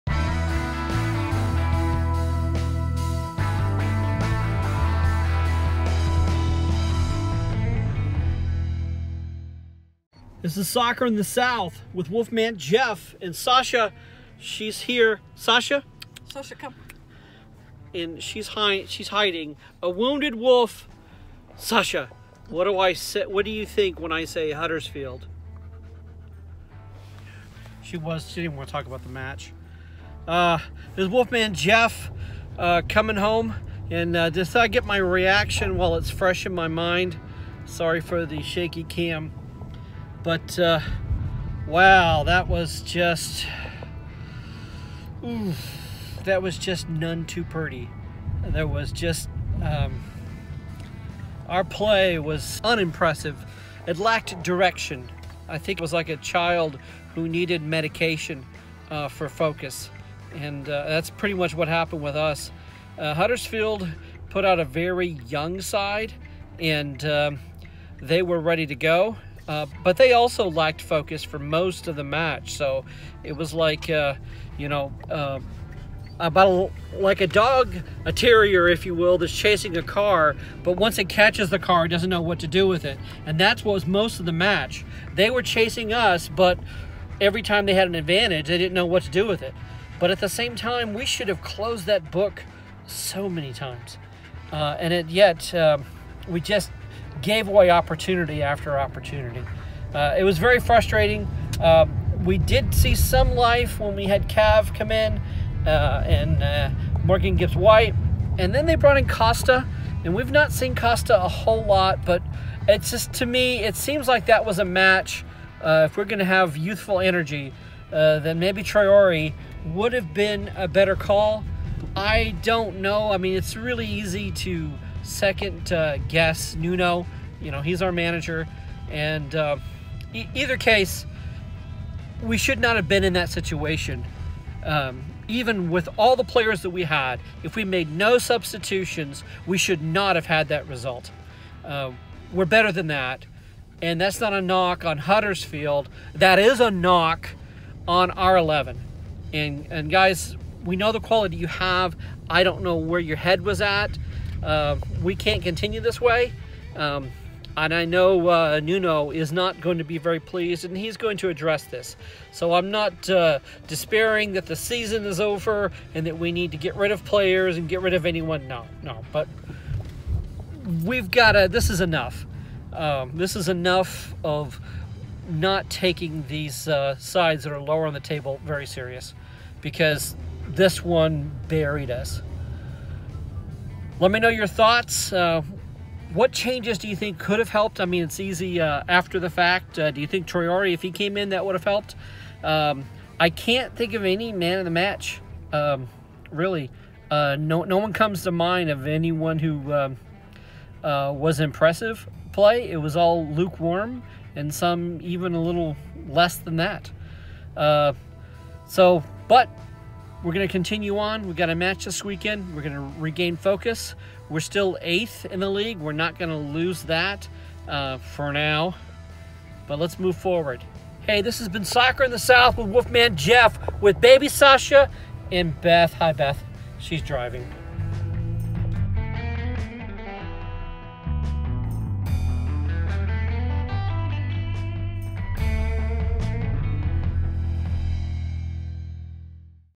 Huddersfield Town V Wolves March Reaction Soccer in the South
Wolfman Jeff, Beth and Sasha react to the Wolverhampton Wanderers loss at Huddersfield Town on 26 Feb 2019. To keep up Soccer in the South news go